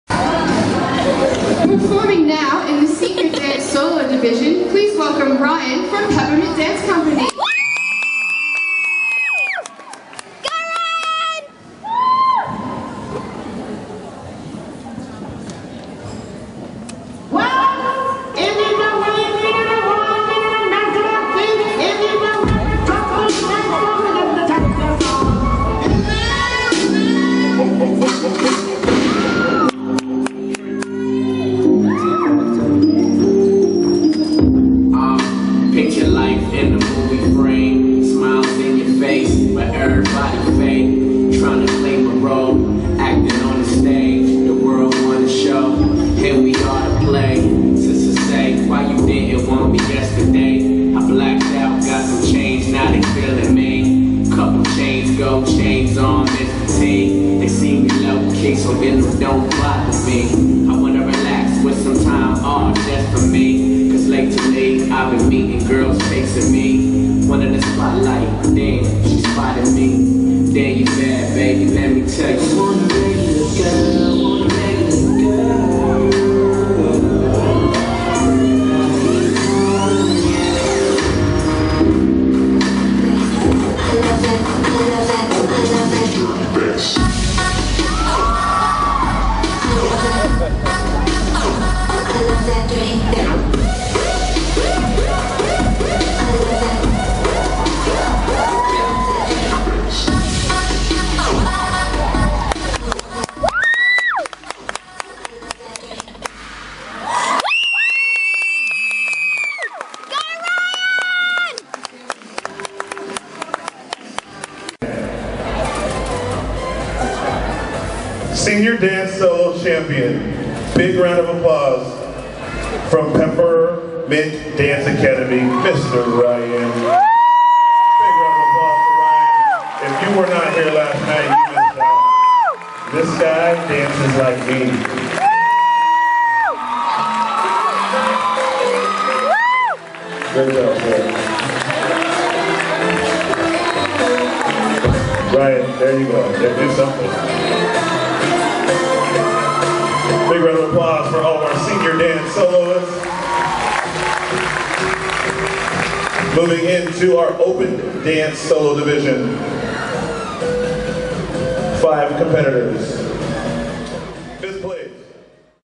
2013 Ryan Stars and Stripes Senior Solo 22092013
Ryan Bell Star And Stripes Senior Hip Hop Solo  Sep 2013  First Place